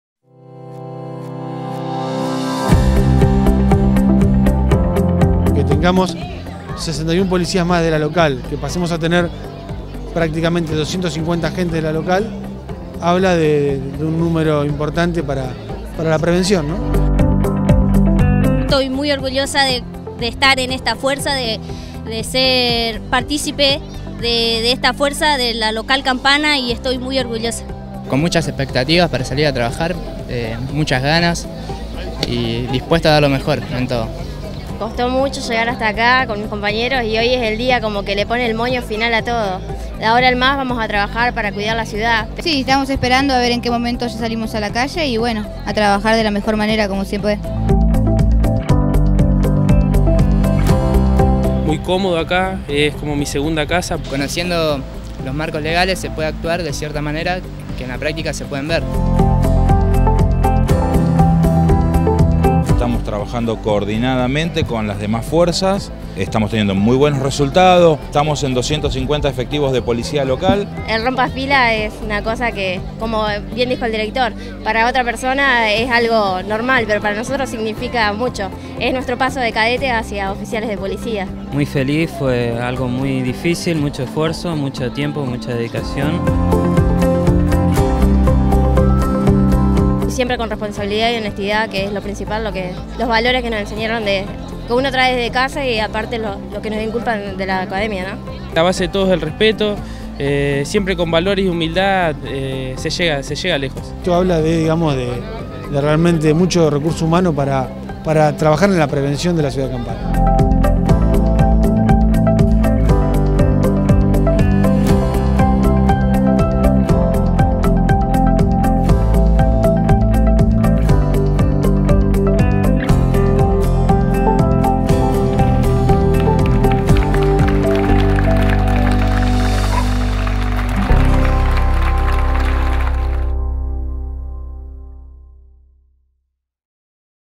Egreso 3º Promoción Policía Local Campana
SUMAMOS 61 POLICÍAS PARA TRABAJAR POR LA SEGURIDAD DE TODOS LOS VECINOS

Con el egreso de la 3ra promoción, ya contamos con 250 agentes policiales en Campana.